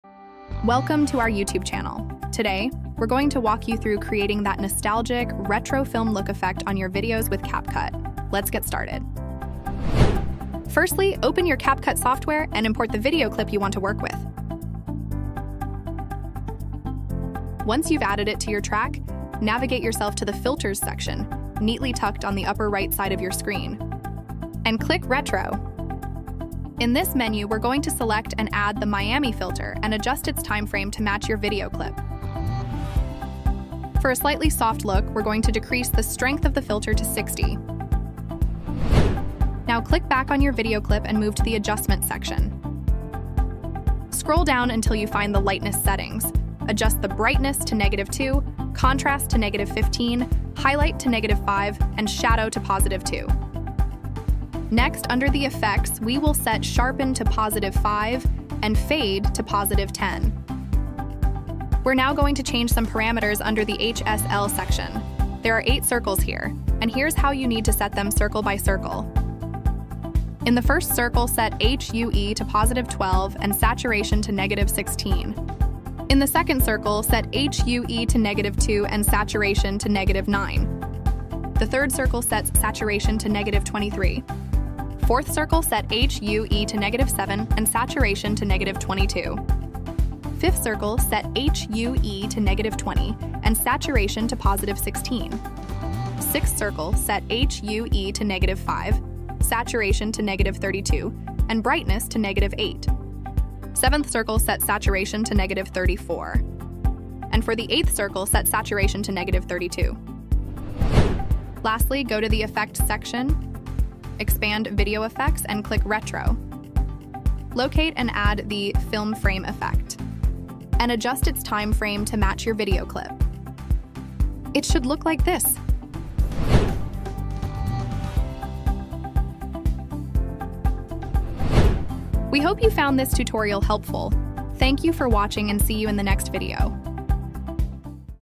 how to make retro film look effect
Learn how to get that vintage, retro film look effect on your video clips using Capcut with our in-depth tutorial. We walk you through each step of the process, from choosing the right ‘retro’ filter to the final touch-ups with the special 'film frame' effect. With tips and tricks that extend beyond the basics, this tutorial is perfect for beginners and advanced users alike.